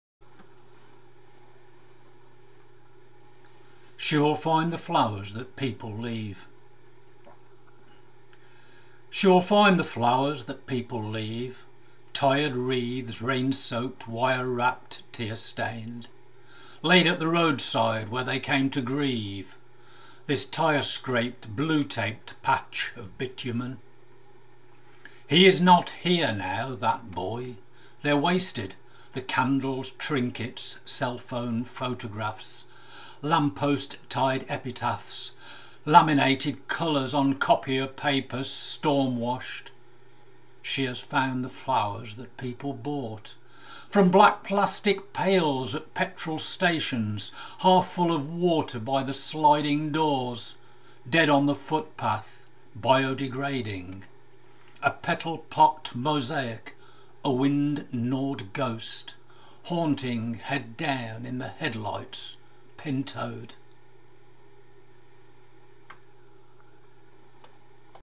She Will Find The Flowers by Joe Reynolds
Poet’s Biography
I am 73 years old and have been writing poetry and short stories for about 60 years. I worked as an engineer until I retired in 2015 when I enrolled on a creative writing course. I have just finished an Open University course on creative writing. I have had some poems and a short story published in local pamphlets.

My poem concerns the floral tributes left at the side of the road after a traffic accident, in particular those involving young people riding 'off-road' motorcycles, and the adulation they receive from their peers; a lot of whom probably did not know the deceased.

Poem Text
She will find the flowers that people leave,
tired wreathes, rain-soaked, wire-wrapped, tear-stained,
laid at the roadside where they came to grieve,
this tyre scraped, blue taped, patch of bitumen.
He is not here now, that boy; they’re wasted,
the candles, trinkets, cell-phone photographs,
lamppost-tied epitaphs, laminated,
colours on copier paper, storm washed.
She has found the flowers that people bought,
from black plastic pails at petrol stations,
half full of water by the sliding doors,
dead on the footpath: biodegrading.
A petal pocked mosaic, a wind gnawed ghost,
Haunting, head down, in the headlights, pin toed.